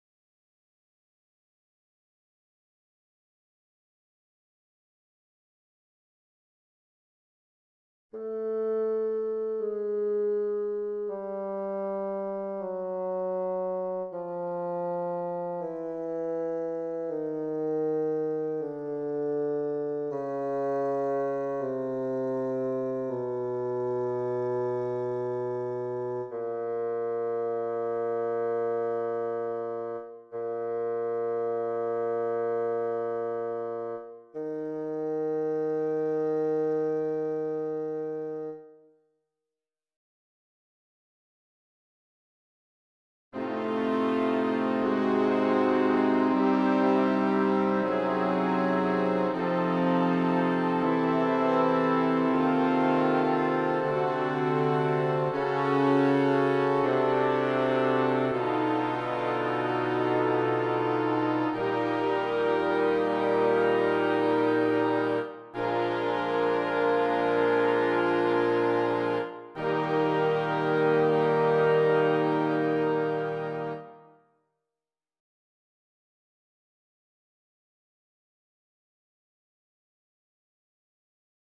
Tuning and Intonation Builder Bassoon 2
Practice your tuning and intonation with these exercises for solo instrument and full band accompaniment.